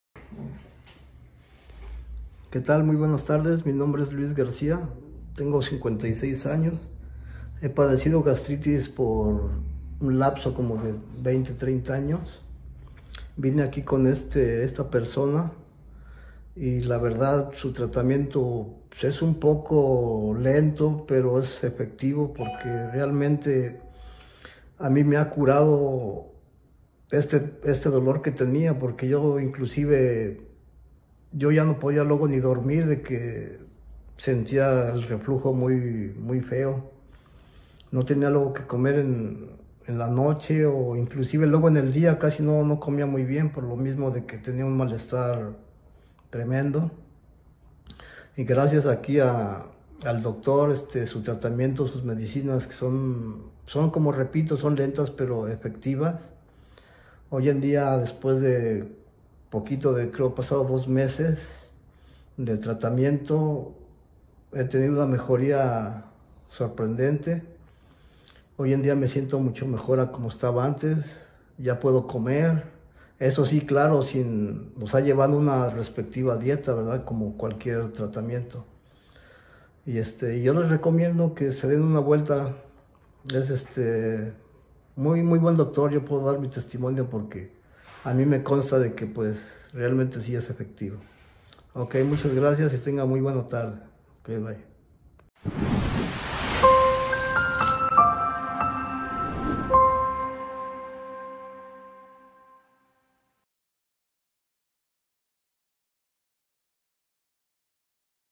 Luis Garcia con el sistema de salud natural Heshoutang
Luis Garcia explicó su experiencia y opinión sobre el sistema de salud natural de Heshoutang